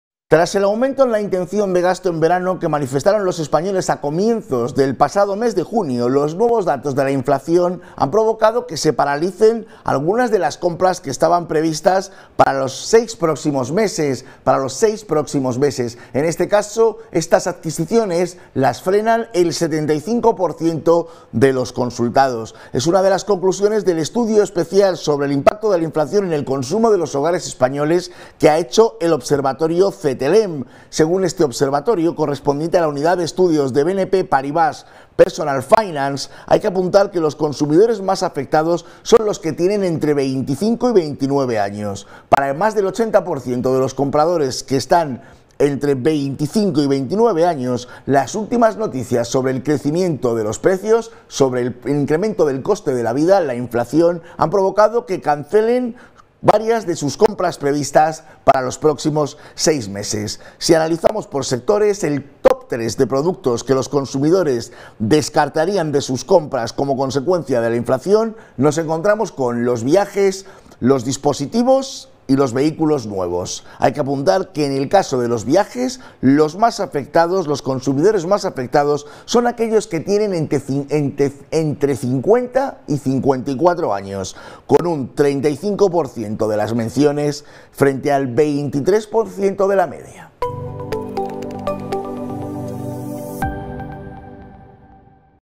El 75% de los españoles descarta realizar compras previstas debido a la inflación
Tras el aumento en la intención de gasto en verano manifestada por los españoles a comienzos del mes de junio, los nuevos datos de la inflación han provocado la paralización de algunas de las compras previstas para los próximos 6 meses por un 75% de españoles.
Esta es una de las conclusiones del estudio especial sobre el impacto de la inflación en el consumo de los hogares españoles de El Observatorio Cetelem, Según el estudio del Observatorio Cetelem, unidad de estudios de BNP Paribas Personal Finance, los consumidores más afectados son el segmento de edad comprendido entre los 25 y 29 años. Para el 83% de los compradores de estas edades, las últimas noticias sobre la inflación, han provocado cancelaciones en sus compras previstas para los próximos 6 meses.
 Si analizamos por sectores, el top 3 de productos que los consumidores españoles descartarían de sus compras previstas como consecuencia del avance de la inflación son: viajes (23%), smartphones (20%) y vehículos nuevos (18%). En el caso de los viajes, los consumidores más afectados han sido aquellos con edades comprendidas entre los 50 y 54 años con un 35% de menciones frente al 23% de la media.